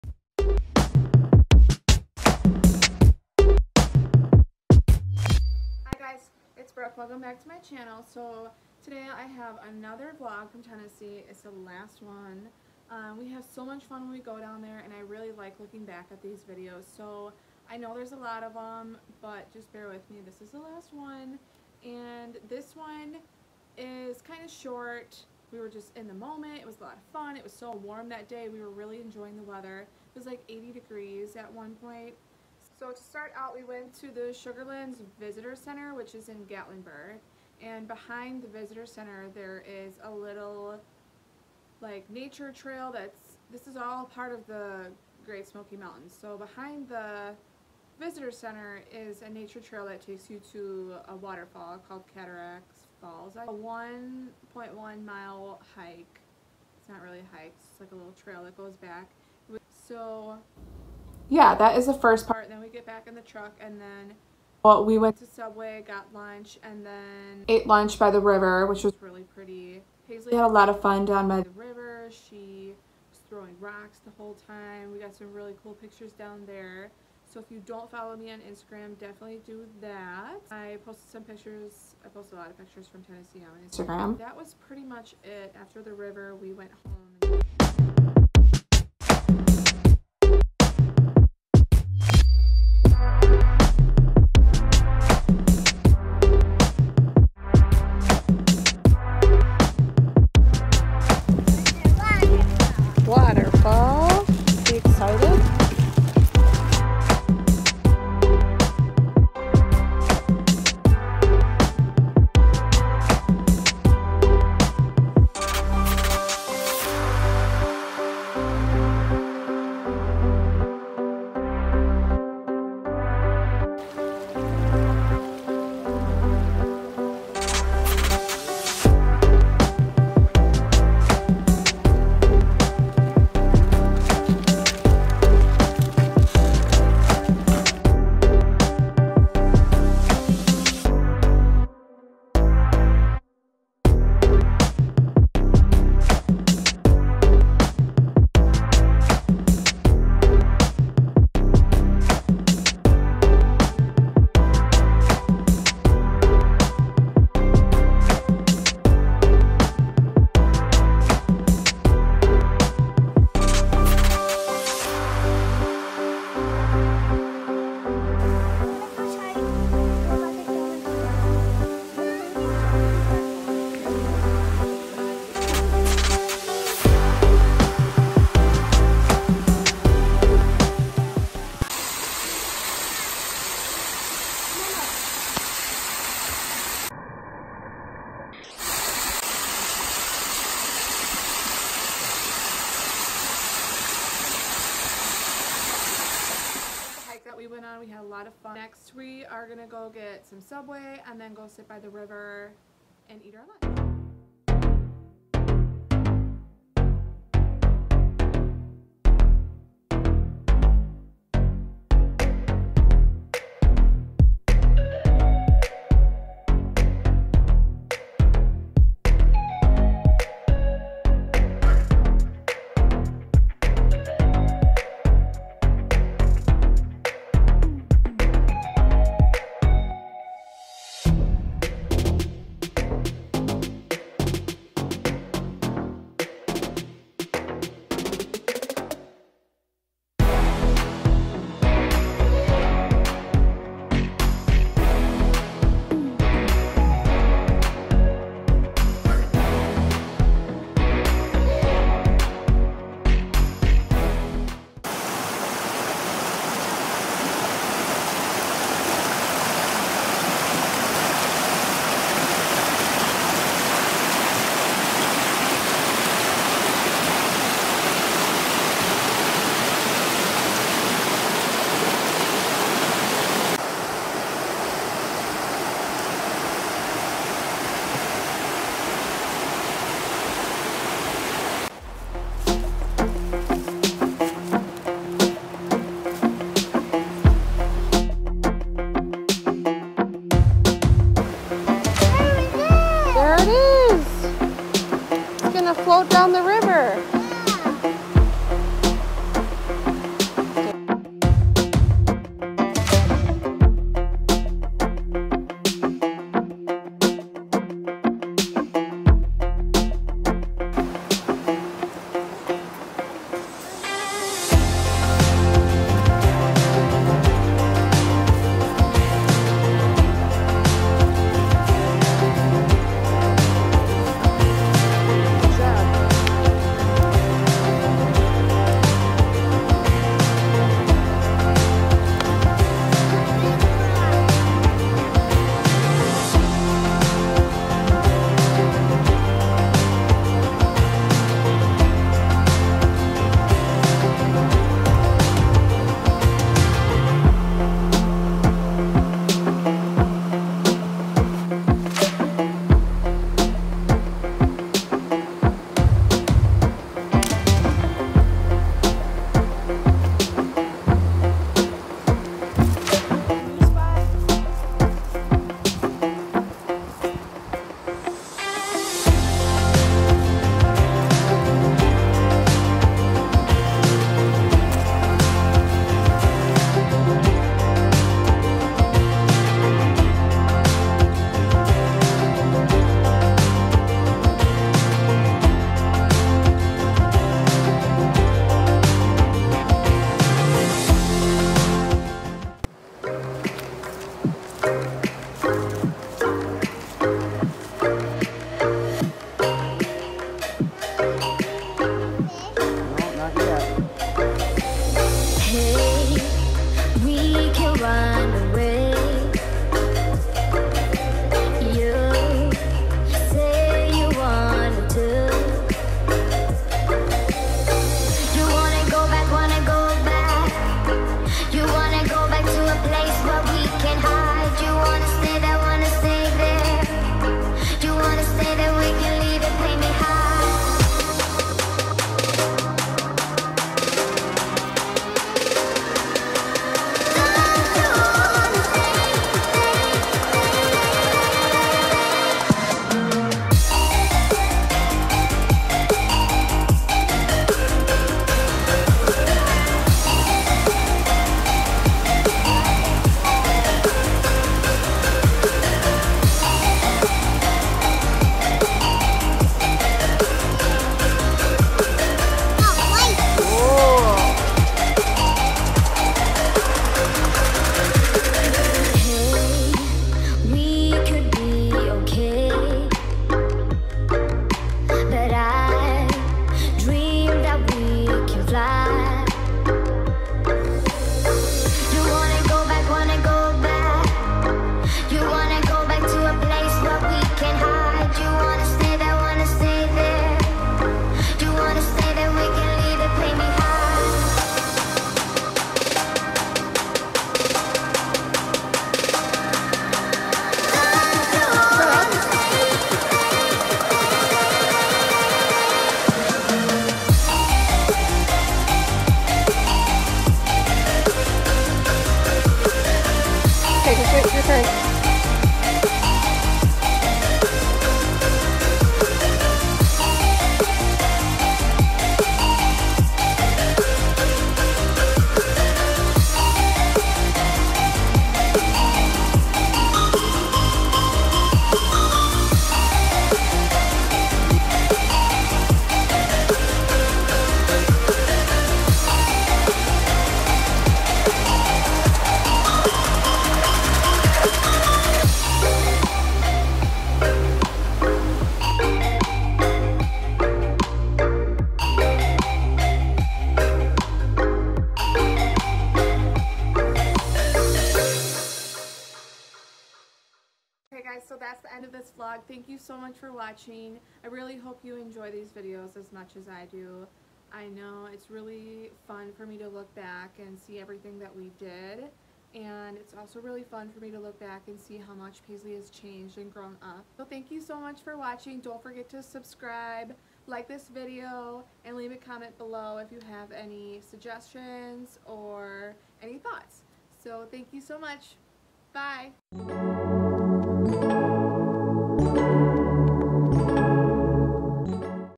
Hiking To A Waterfall | Lunch With A View | Brooke Brewer
Hey Guys! Today's video is the last vlog from our Tennessee trip over the Holidays. We went for a 1.1 mile "hike" and saw cataract falls, then we got lunch from subway and ate next to the river! Then we went to the last waterfall of the day which was amazing.